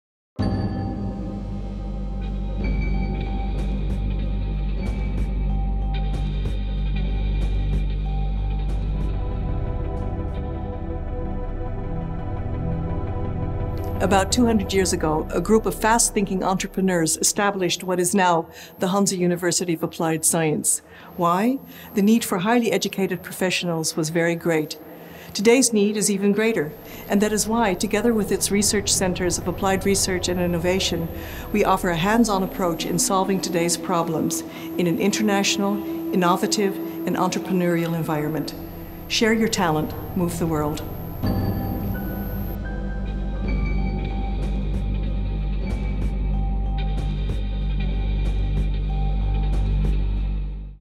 Introduction to Hanze University of Applied Sciences
Welcome to Hanze!
Hanze UAS is situated in Groningen, the Netherlands and is part of the Geert Hofstede Consortium
The consortium offers a Joint Master International Communication
Film by Siegmund Audiovisuele Produkties.